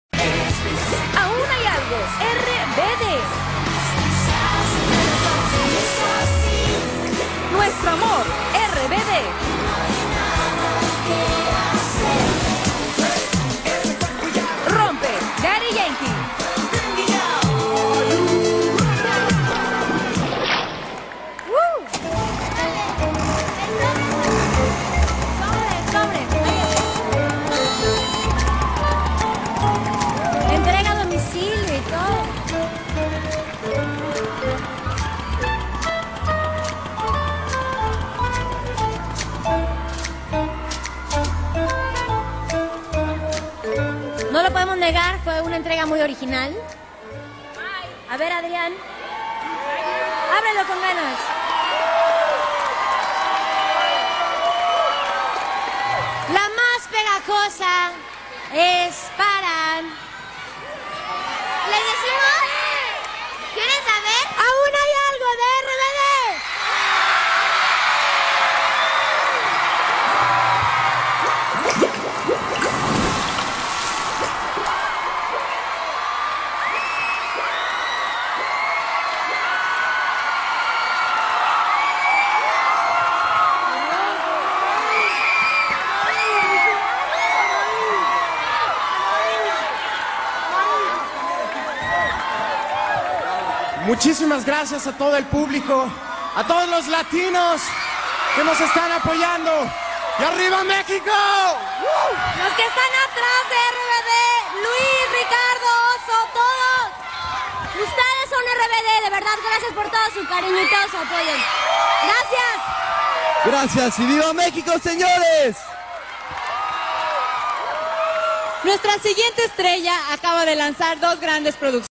PJ 2006-RBD vyhralo v kategórii ,,La canción más pegajosa"
Premios Juventud 2006 kategória ,,La canción más pegajosa"-Nachytľavejšia pieseň